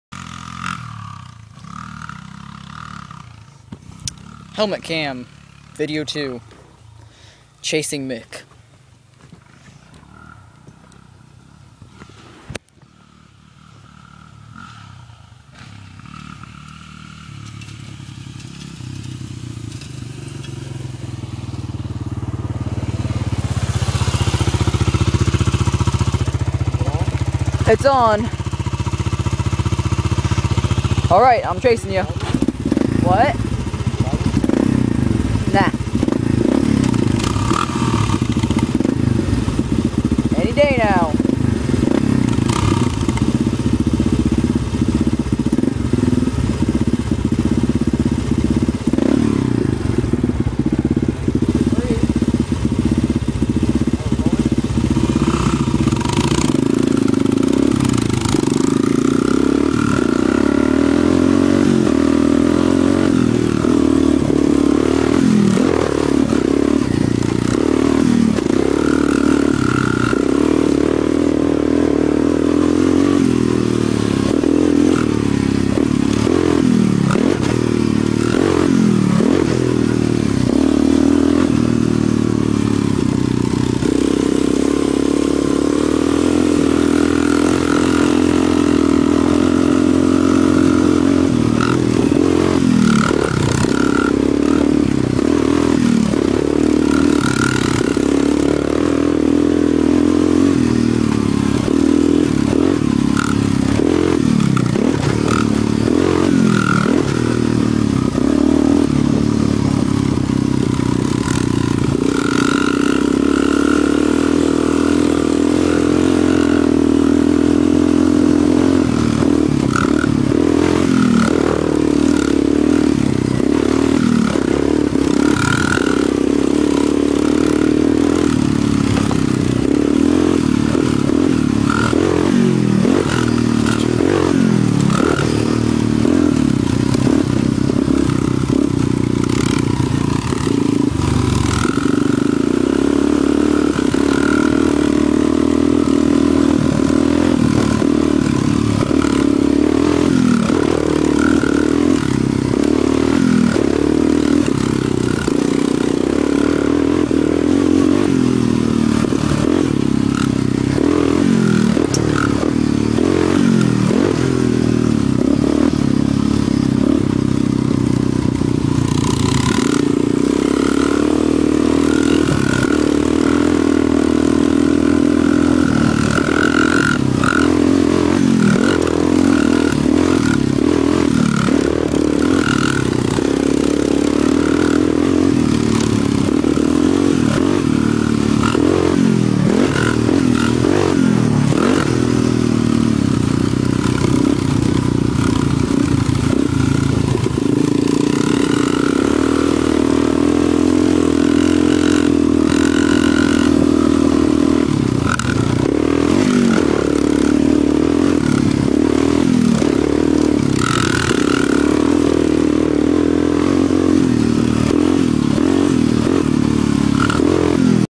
Helmet cam. Me chasing mick
Me on my yamaha raptor 350 chasing Mick on his Honda trx 250. Helmet cam.